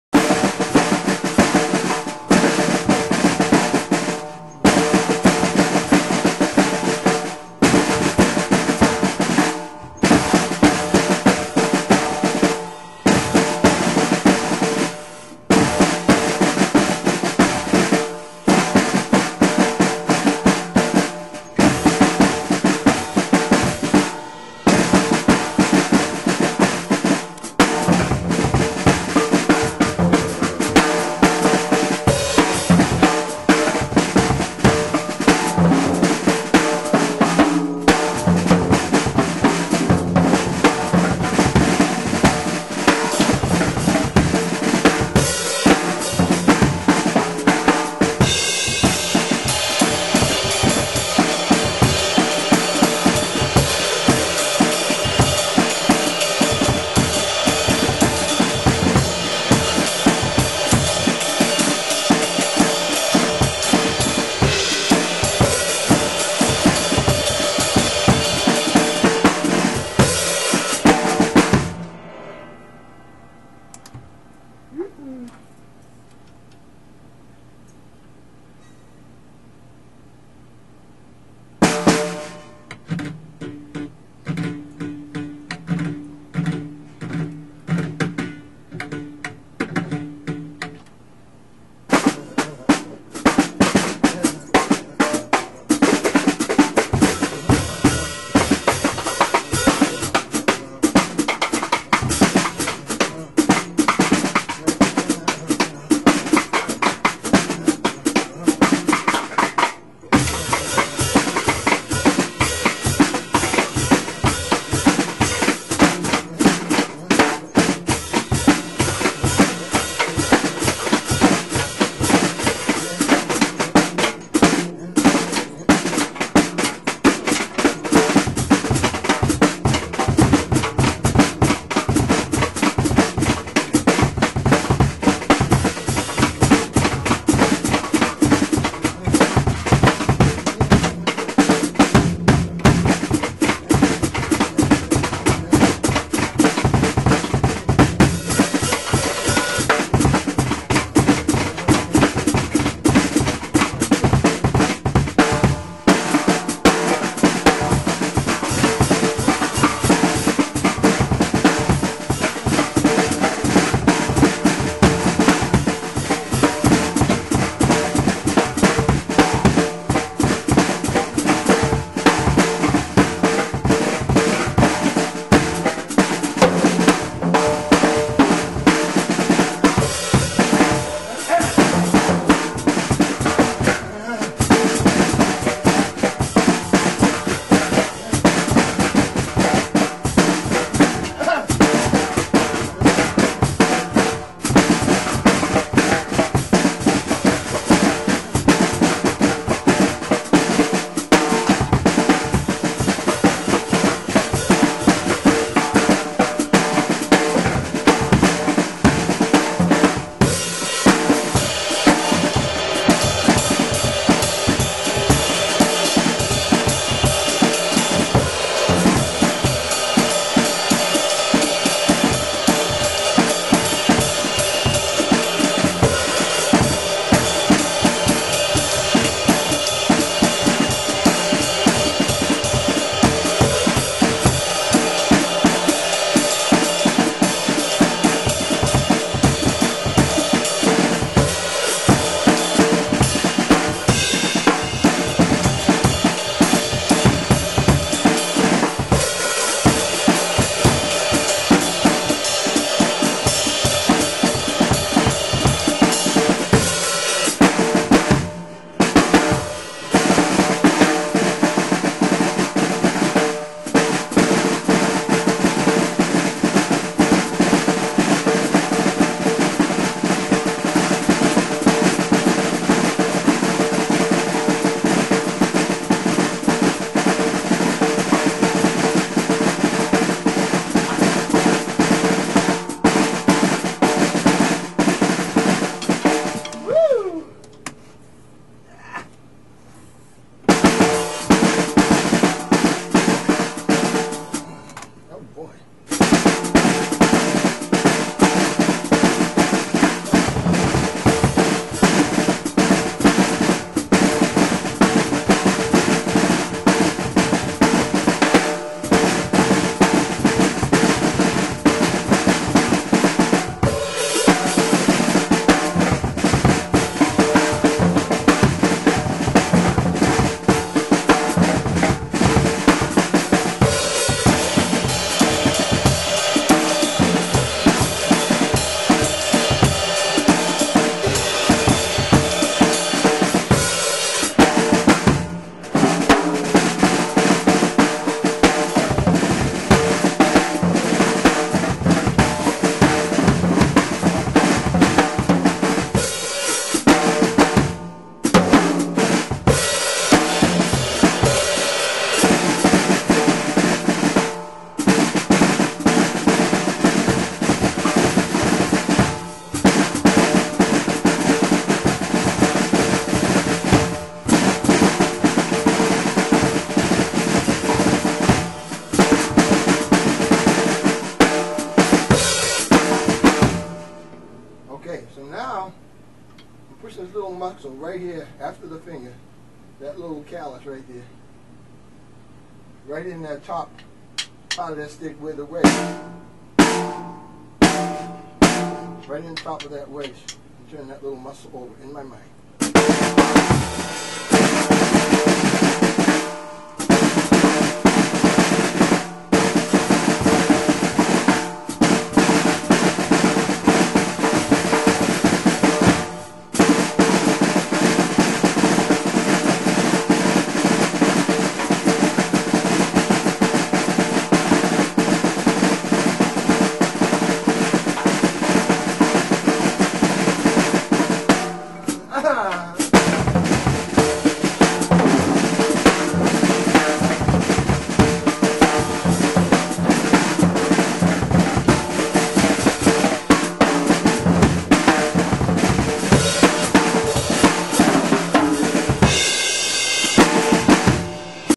you can create many levels of challenge with cross rim ideas-LN.wmv
playing cross rim is also a lost art like stickometrics and hihatetrics.lenny nelson